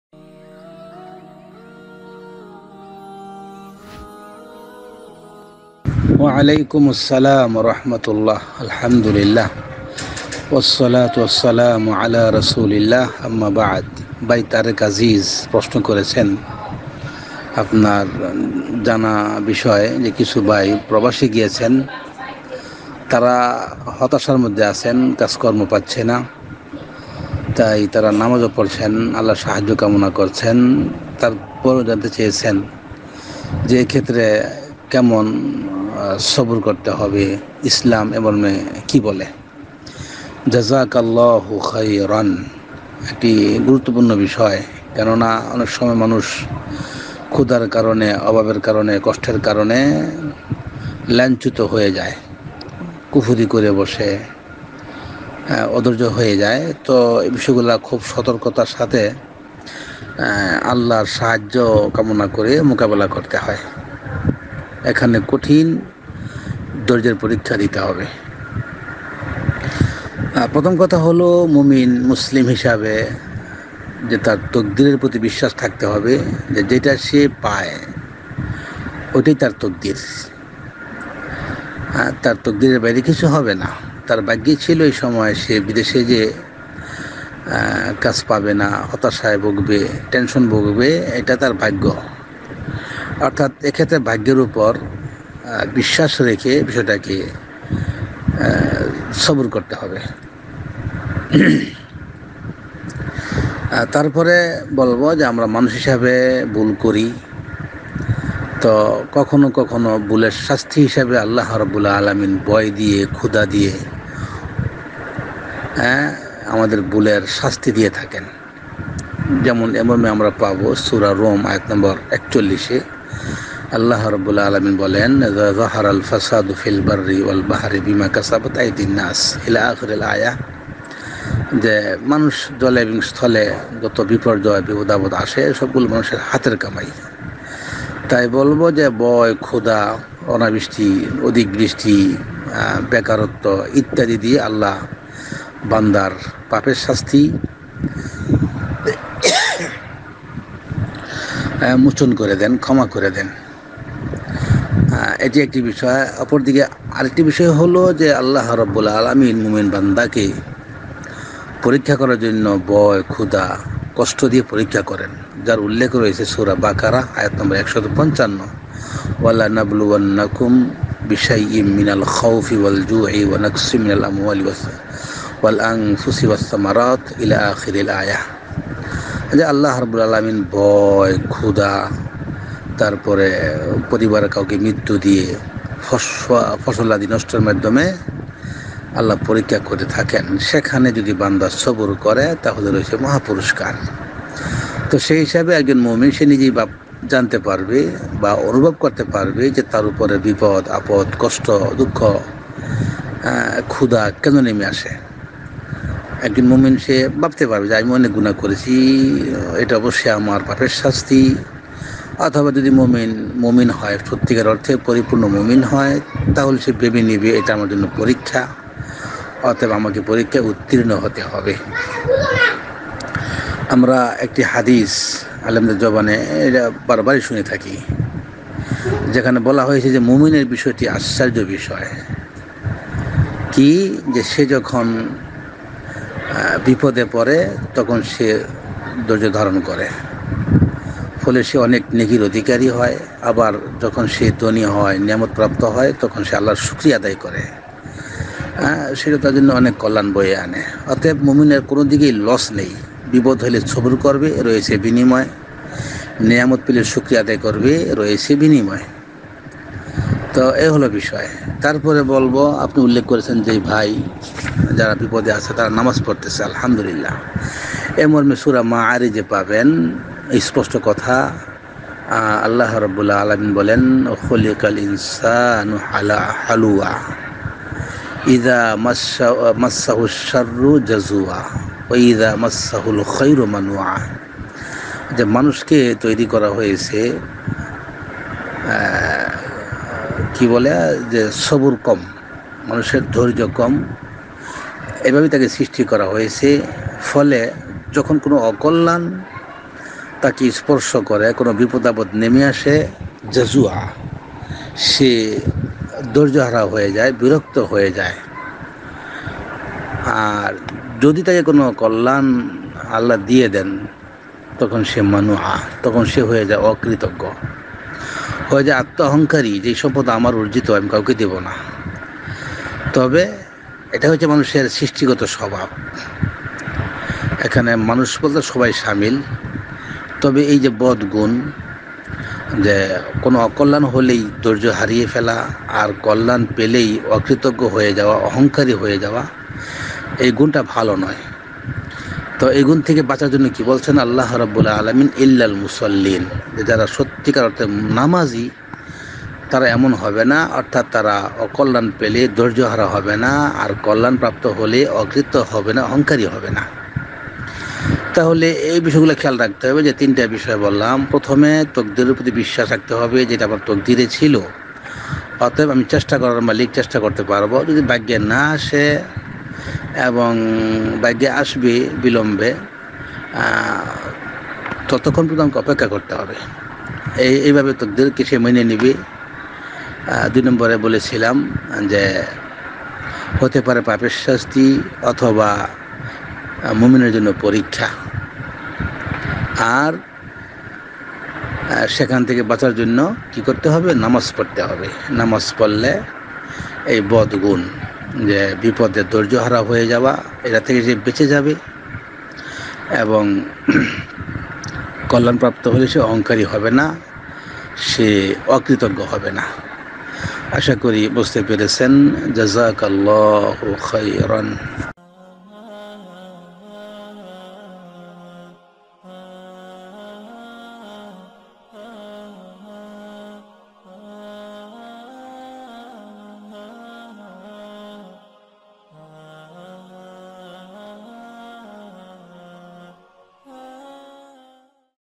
ধৈর্য সবর প্রসঙ্গে ইসলাম কি বলে ᴴᴰ┇শাইখ ইসহাক বিন ইব্রাহিম Islamic Question
Present by.
Al Huda Wan Noor Foundation
প্রশ্নঃ- ধৈর্য সবর প্রসঙ্গে ইসলাম কি বলে
উত্তর দিয়েছেনঃ- শাইখ ইসহাক বিন ইব্রাহিম
◆◆◆ Chapters Added ◆◆◆
0:00 Introduction
0:06 Main Part
6:55 End Introduction